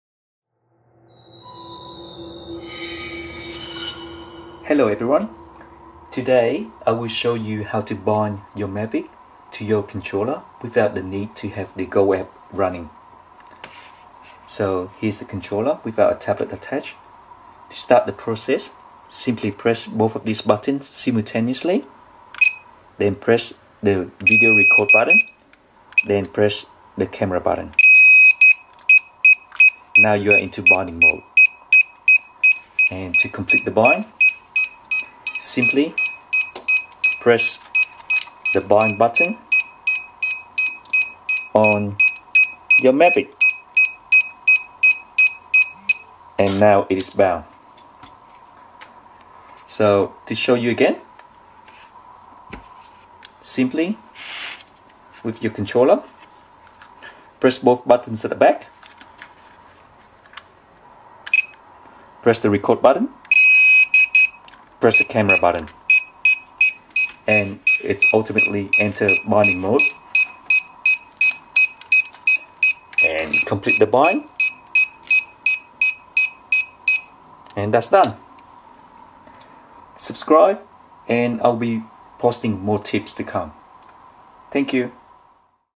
DJI MAVIC PRO PLATINUM - HOW TO LINK REMOTE CONTROL WITHOUT THE GO4 APP OR TABLET / SMARTPHONE
Simple and easy way to quickly bind your DJI Mavic RC without the need to have the Go App or tablet or smartphone attached.